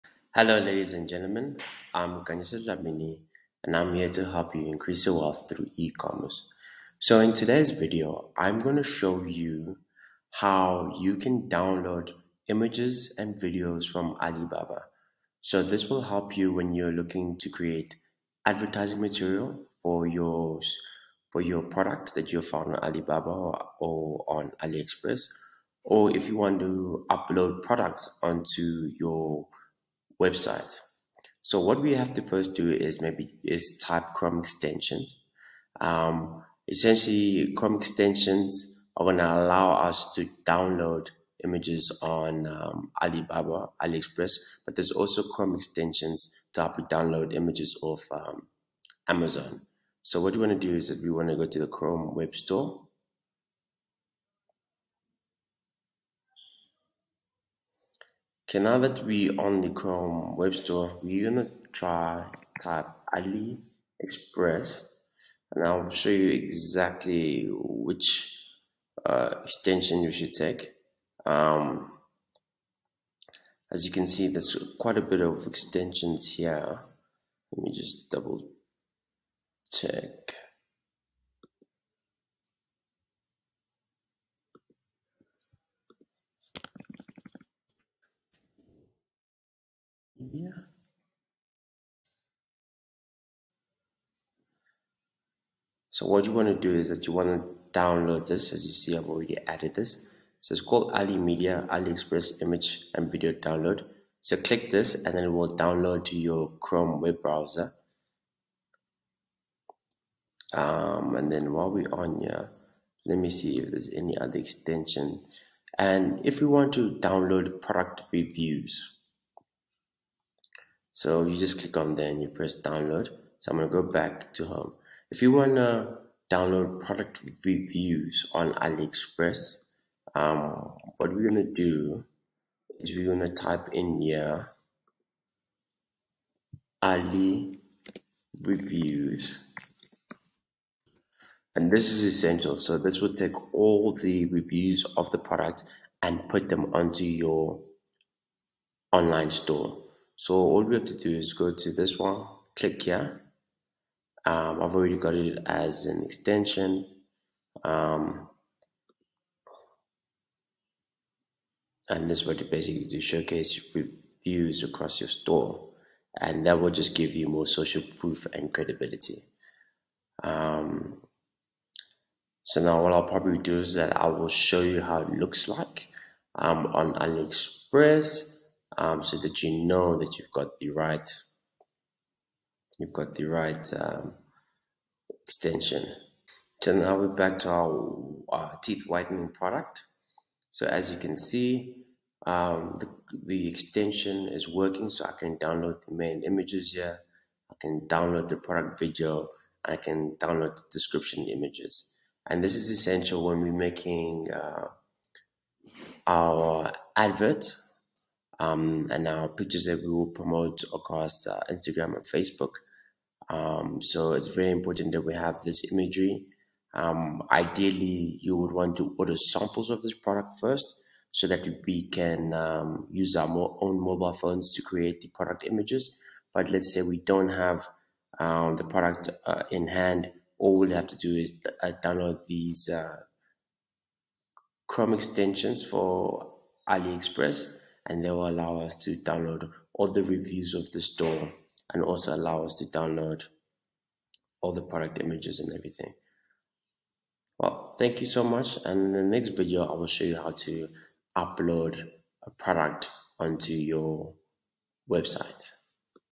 How to download product images from Alibaba & Aliexpress.
"Are you looking to source products from Alibaba or Aliexpress? In this tutorial, we'll show you how to download product images from these popular websites. From finding the right products to saving and organizing your images, we'll cover all the essential steps you need to take. By the end of this video, you'll have a solid understanding of how to find and download product images from Alibaba and Aliexpress.

Visit: Millionrandmoves.co.za